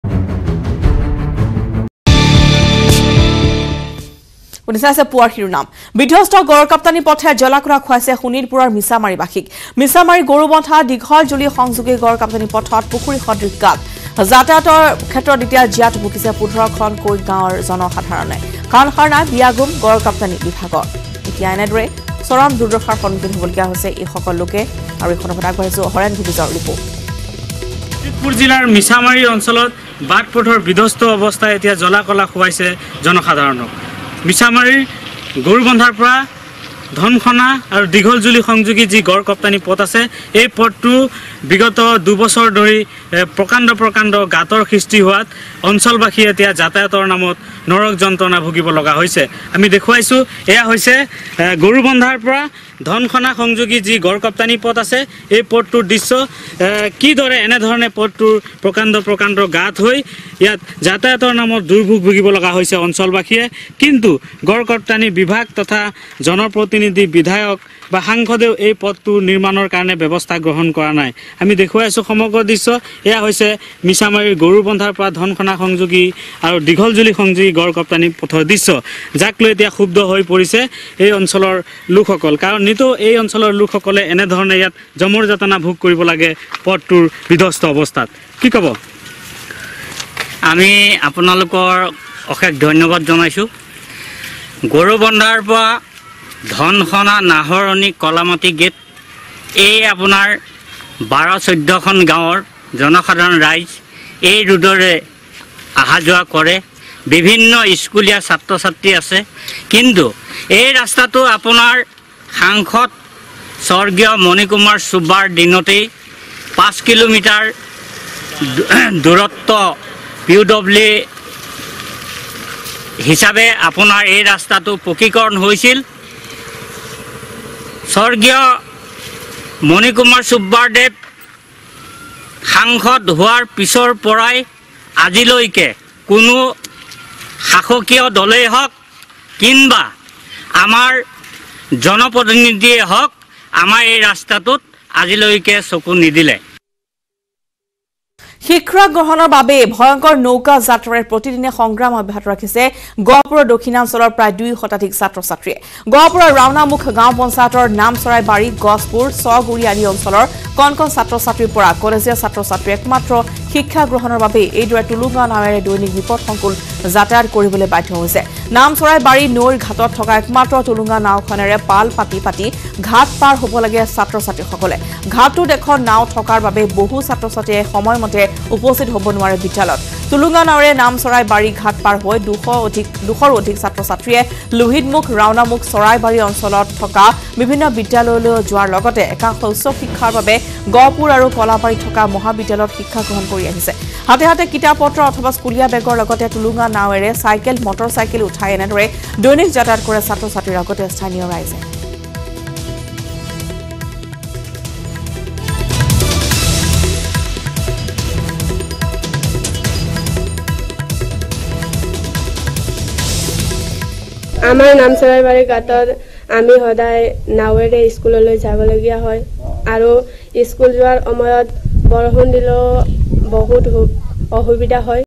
Sonitpurৰ মিছামাৰী বাসীৰ দুৰ্দশা
মিছামাৰীবাসীৰ যাতায়ত যন্ত্ৰনা। চাওক এই প্ৰতিবেদন ।